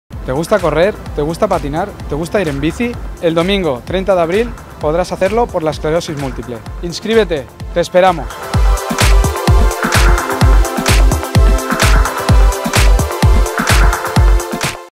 Tamudo es mou per l'esclerosi múltiple
Raúl Tamudo se solidaritza amb l'esclerosi múltiple i anima a tothom a participar al Mou-te per l'esclerosi múltiple ja sigui pedalant, corrent o anant en bicicleta al Circuit de Barcelona-Catalunya el 30 d'abril. No t'ho perdis! Mou-te!